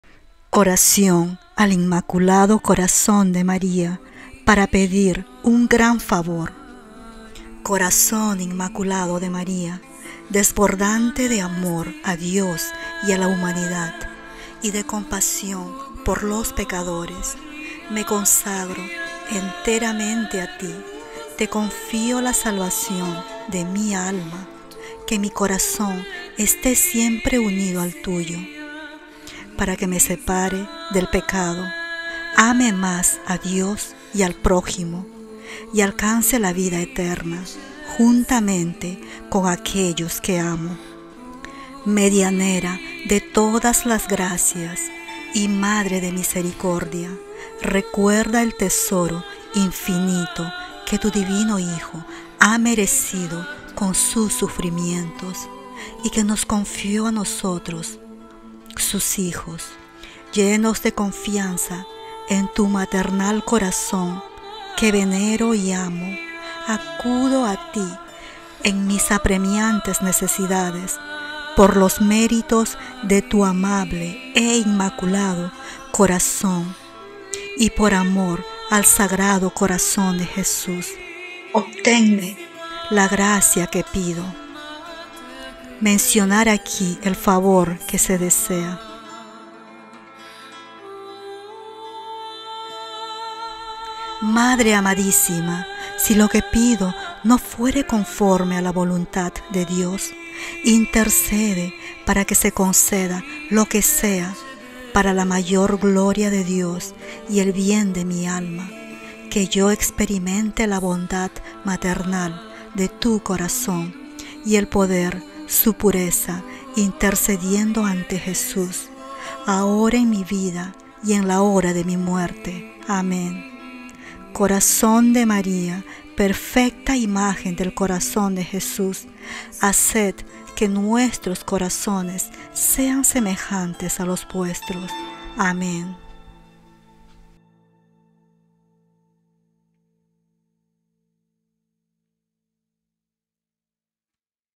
NECESITAS UN FAVOR URGENTE?, PIDE AL INMACULADO CORAZÓN DE MARÍA | ORACIÓN
En urgentes necesidades puedes solicitar ayuda divina al Inmaculado Corazón de María; puedes pedir un gran favor con esta milagrosa oración.
“Mi Inmaculado Corazón será tu refugio y el camino que te conducirá a Dios”, son las palabras con las que la Virgen María recuerda que a ella se puede acudir para encontrar y fortalecer la fe, y no es para menos, pues Ella ha demostrado que su corazón arde de amor Divino y que, además, puede remitirnos también al Sagrado de Corazón de Jesús, pues ella y su Hijo están ligados para toda la eternidad.

De hecho, el Inmaculado Corazón de María ha sido, desde hace mucho tiempo, la más poderosa adoración a la Madre de todos en el catolicismo, al demostrarla como la forma más segura de llegar a Jesús, no solo limitándose al plano físico, sino a su propia alma en la que se resguardan todas sus virtudes, teniendo en cuenta que no solo hizo parte del proceso del Niño Dios en el la gestación, sino también en su crianza, la misma que es reflejo de amor y pureza.

Es por esta razón que son muchas las personas que suelen hacer sus peticiones especiales al Inmaculado Corazón de María en diferentes situaciones, en especial cuando atraviesan por urgentes y desesperadas necesidades.